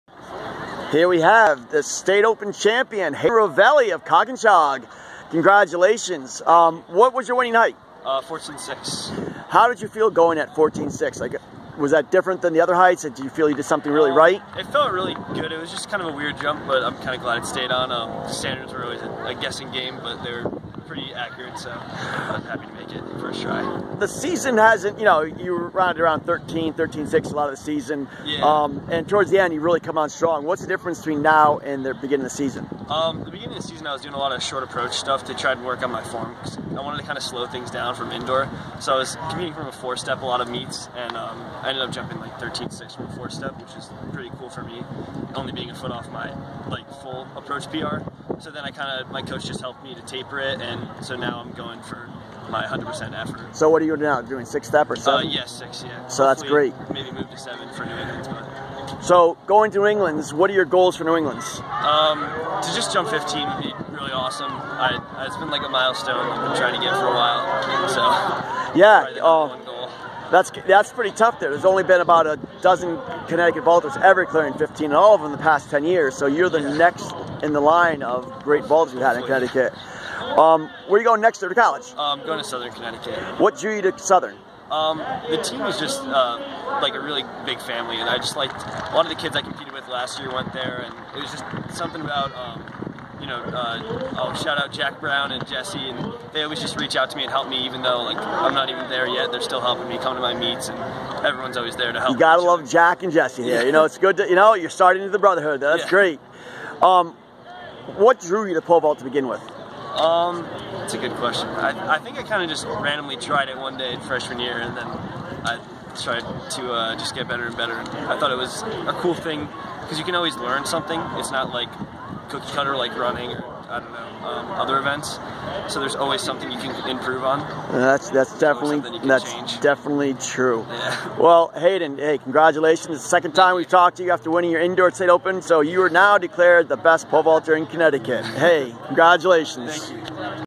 State Open Pole Vault Champion Hayden Rovelli of Coginchaug
Coginchaug's Hayden Rovelli sets a PR of 14'6 to win the CIAC State Open in the pole vault.  Interview by Marty Ogden